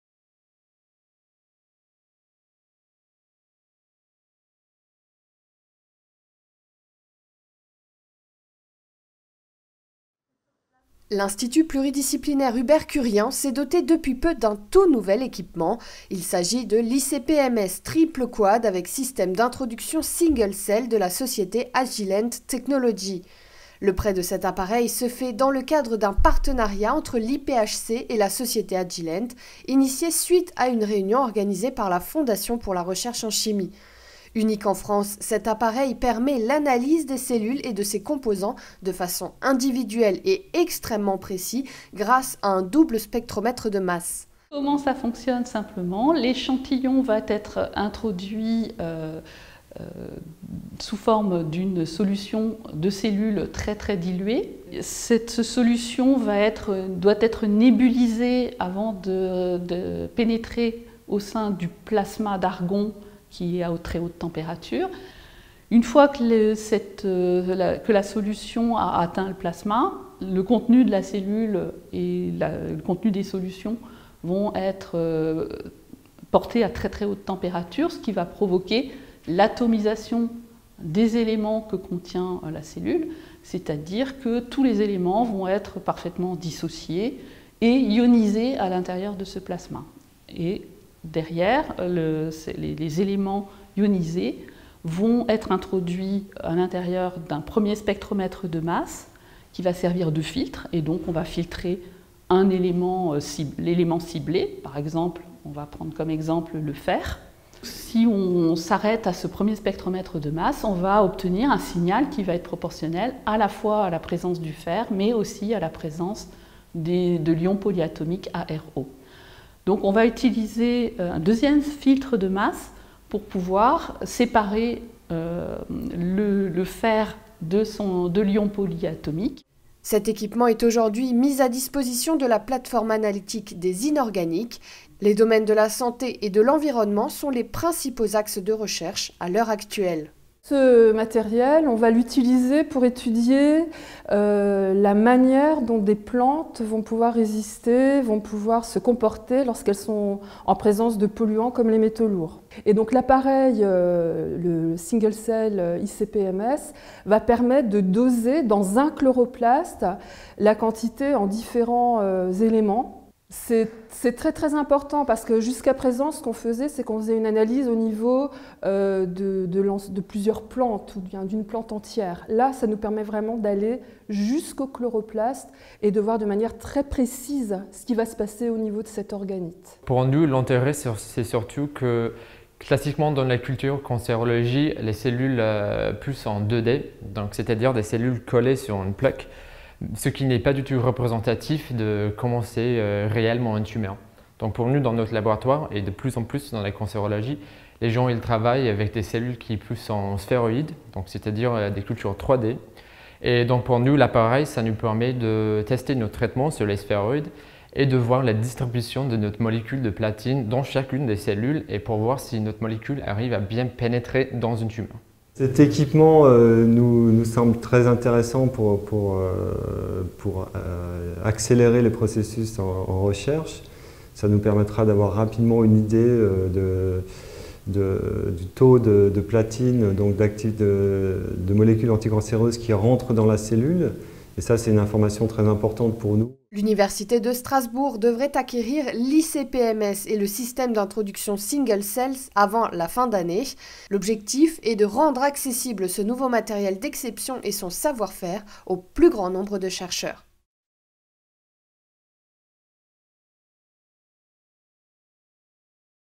Un nouvel équipement à l'Institut pluridisciplinaire Hubert Curien
L'Institut pluridisciplinaire Hubert Curien s'est doté d'un nouvel équipement prêté par la société Agilent Technologies. L'ICP-MS (spectrométrie de masse à plasma induit) permet la caractérisation des nanoparticules dans l’environnement et les milieux biologiques à l'échelle d'une cellule unique.

Retrouvez toute l'actualité de la recherche à l'Université de Strasbourg